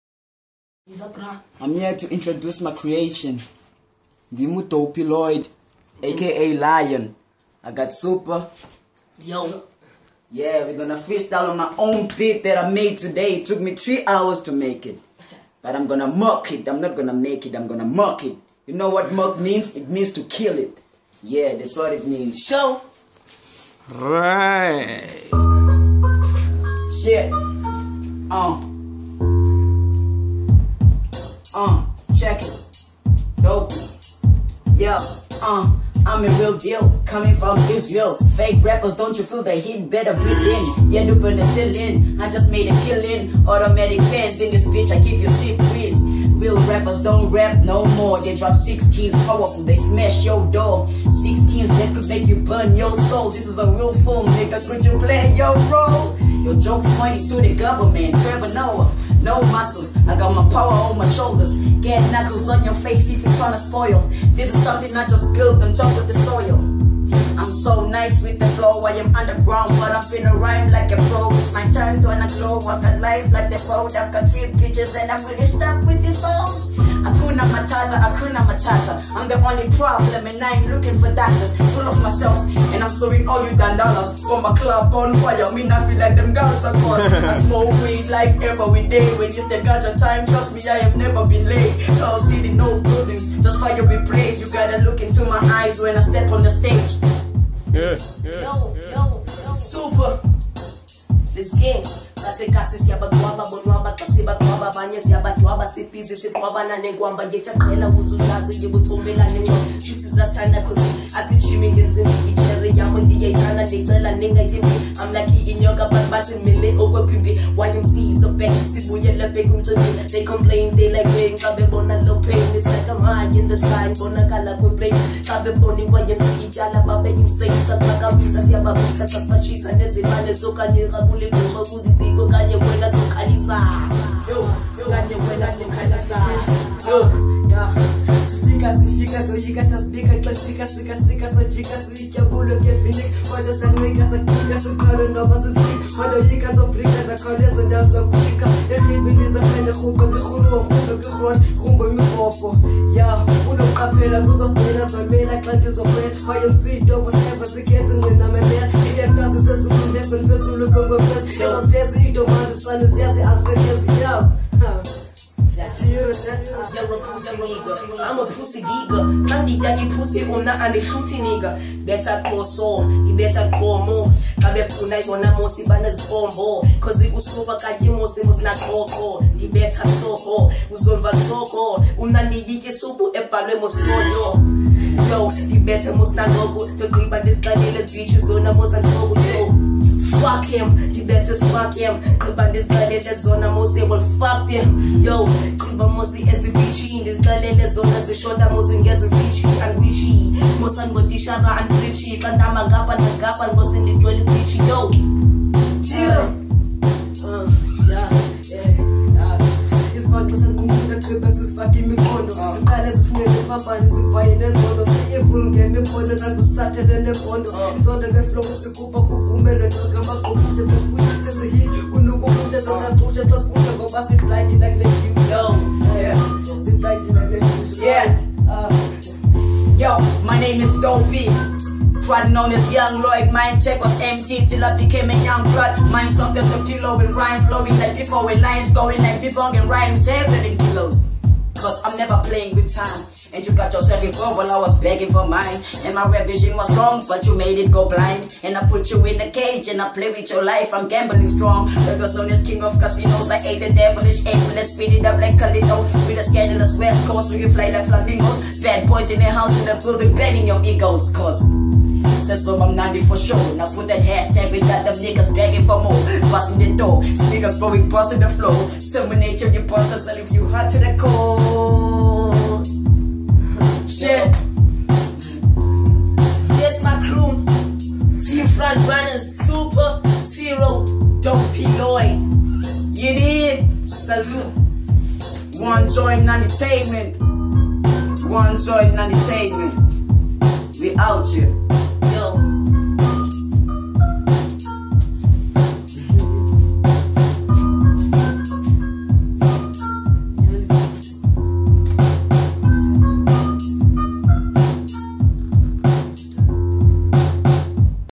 Lion Dopie Lloyd feat. Super (Athi) & Thirow (Freestyling)
Checkout this young cats, killing this beat that one of them recently made. Lion Lloyd (17) Super Athenkosi Dulwana (14) and Thirow Johnson (15)